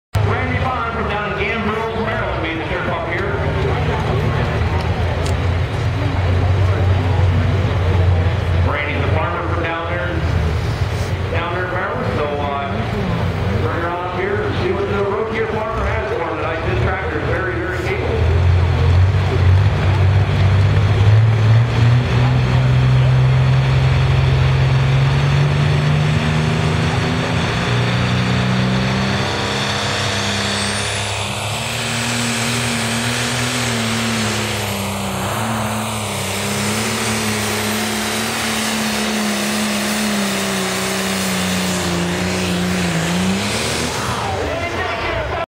The Road Gear Farmer - Case IH Tractor Pull
Harford Fair 2012 Tractor Pulls